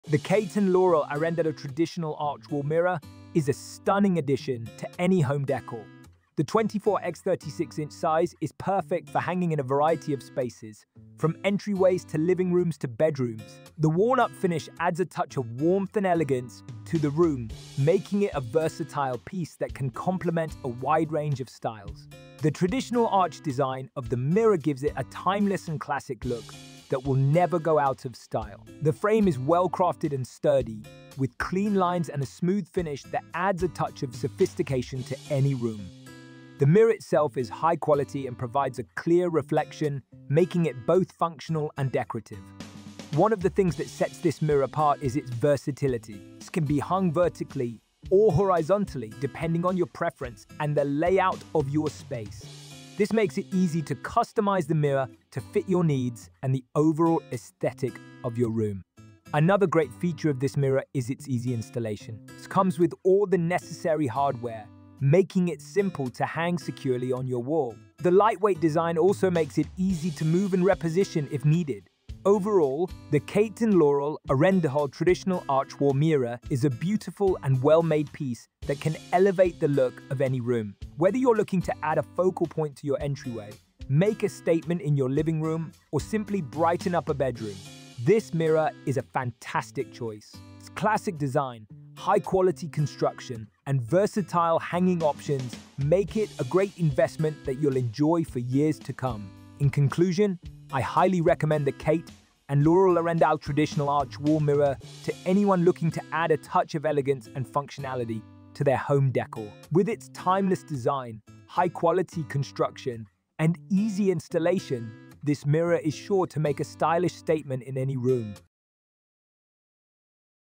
Kate and Laurel Arendahl Traditional Arch Wall Mirror 24 x 36 Walnut Review
The Kate and Laurel Arendahl Traditional Arch Wall Mirror is a stunning addition to any home decor. The 24 x 36 inch size is perfect for hanging in a variety of spaces, from entryways to living rooms to bedrooms. The walnut finish adds a touch of warmth and elegance to the room, making it a versatile piece that can complement a wide range of styles.

The traditional arch design of the mirror gives it a timeless and classic look that will never go out of style. The frame is well-crafted and sturdy, with clean lines and a smooth finish that adds a touch of sophistication to any room. The mirror itself is high-quality and provides a clear reflection, making it both functional and decorative.

One of the things that sets this mirror apart is its versatility. It can be hung vertically or horizontally, depending on your preference and the layout of your space. This makes it easy to customize the mirror to fit your needs and the overall aesthetic of your room.

Another great feature of this mirror is its easy installation. It comes with all the necessary hardware, making it simple to hang securely on your wall. The lightweight design also makes it easy to move and reposition if needed.

Overall, the Kate and Laurel Arendahl Traditional Arch Wall Mirror is a beautiful and well-made piece that can elevate the look of any room. Whether you’re looking to add a focal point to your entryway, make a statement in your living room, or simply brighten up a bedroom, this mirror is a fantastic choice. Its classic design, high-quality construction, and versatile hanging options make it a great investment that you’ll enjoy for years to come.

In conclusion, I highly recommend the Kate and Laurel Arendahl Traditional Arch Wall Mirror to anyone looking to add a touch of elegance and functionality to their home decor. With its timeless design, high-quality construction, and easy installation, this mirror is sure to make a stylish statement in any room.